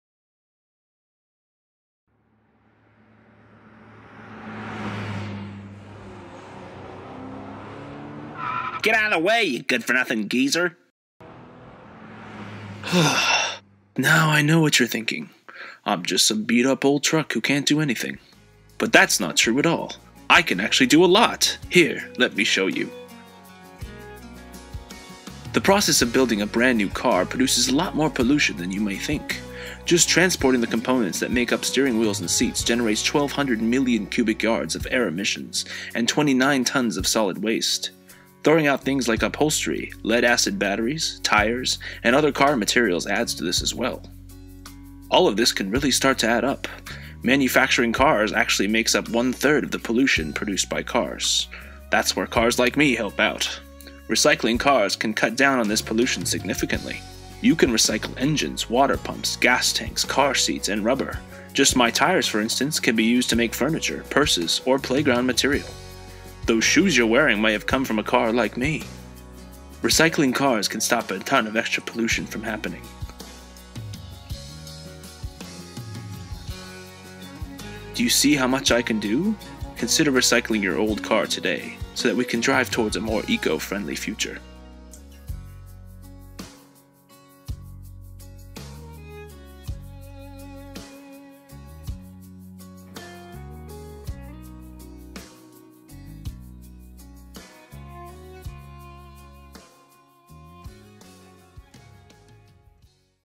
2015 JASON Youth Video and Poster Contest Winner: Georgina Cahill Driving Towards a Better Future
Georgina Cahill is the Grand Prize Winner of JASON Learning and ISRI"S 2015 Youth Video and Poster Contest. Georgina’s nearly two minute video uses clay animation to showcase the positive impact recycling a car has on the environment.